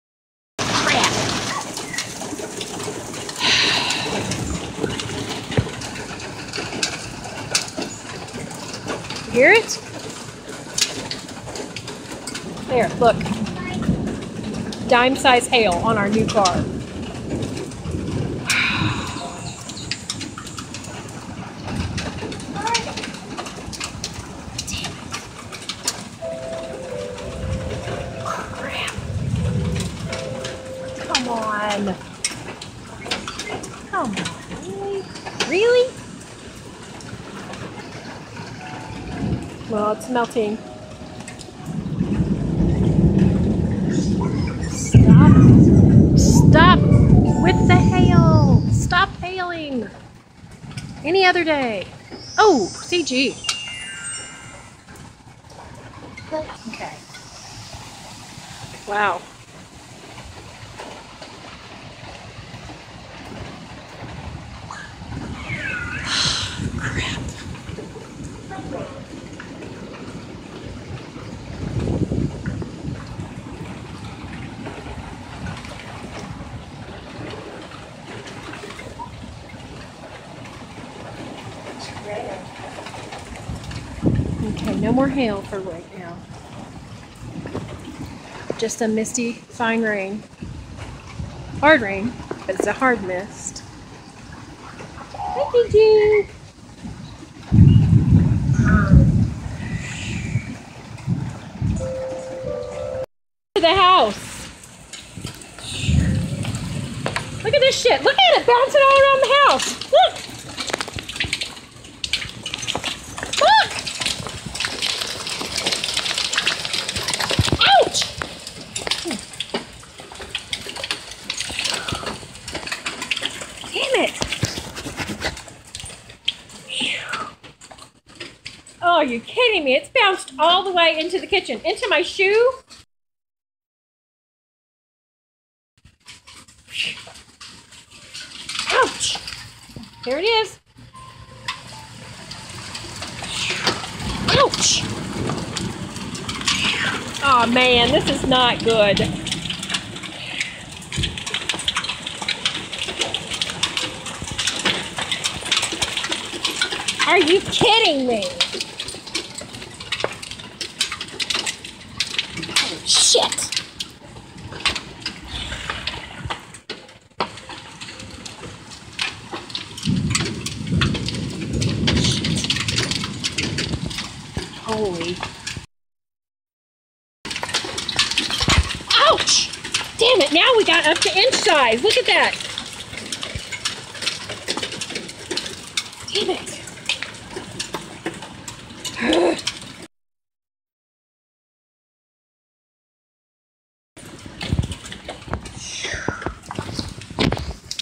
Hail in Miamitown, Ohio 08- 03-15
Video of the storm that occurred in Miamitown, Ohio. My wife was "concerned" that it would damage our new car we got just days ago.
Luckily, no damage to the car. Max size of the hail was half-Dollar